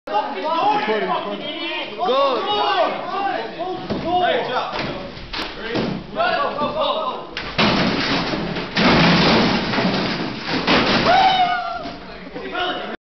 bombs away
just another day at rambam highschool of throwing chairs and desks and making lots of noise